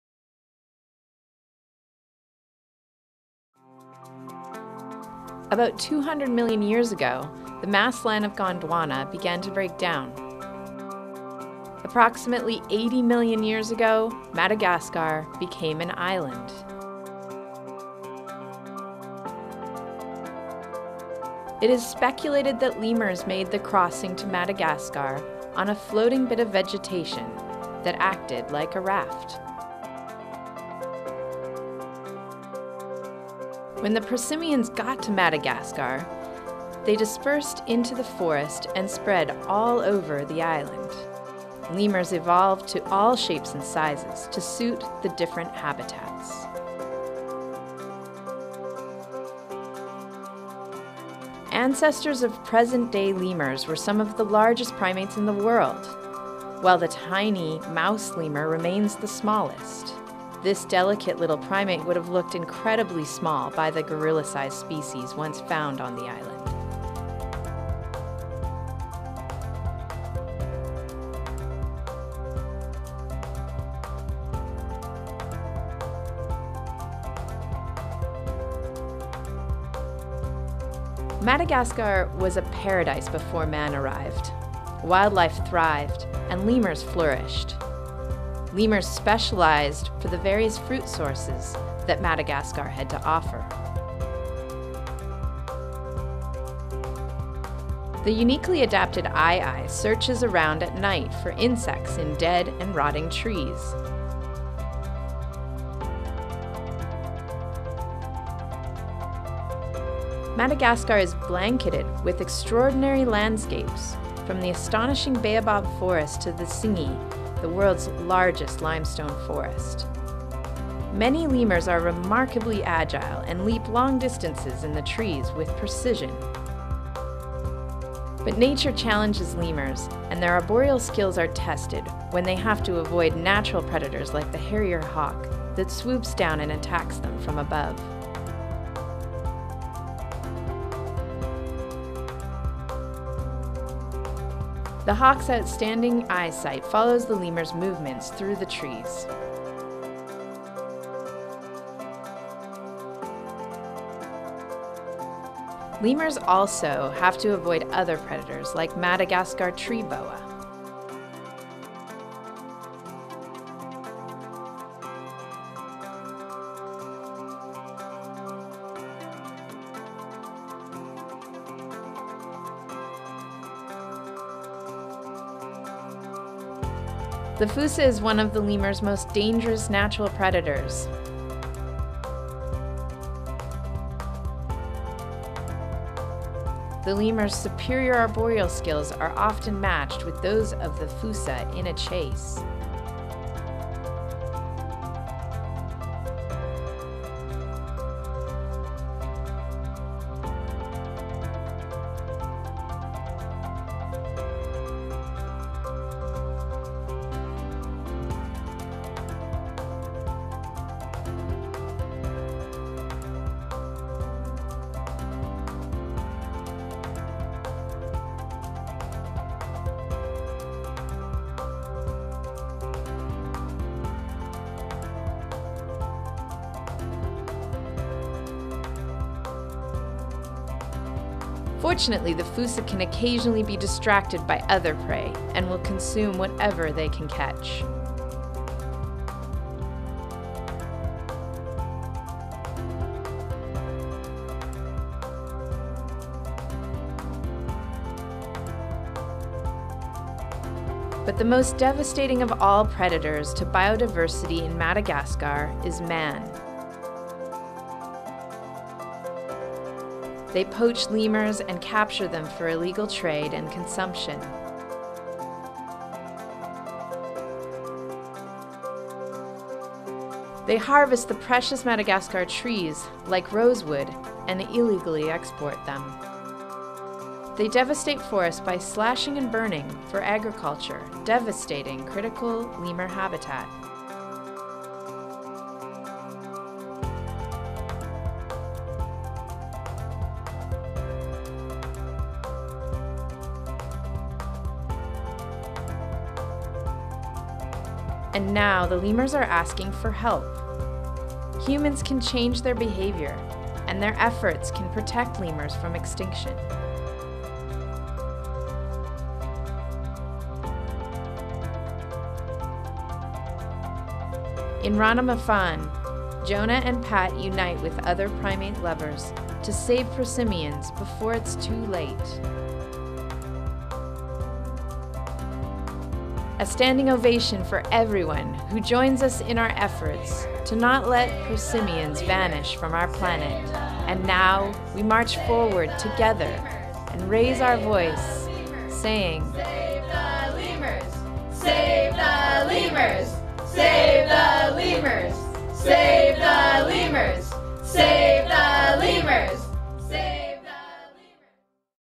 Save The Lemurs of Madagascar - 2013 Prosimian Congress Video
94% of Lemur species are Endangered.  Habitat loss and hunting/poaching are the primary causes, Lemurs loose 2 million hectares of forest each year.  There is a new 3 year action plan to help protect them and their habitat but we need help.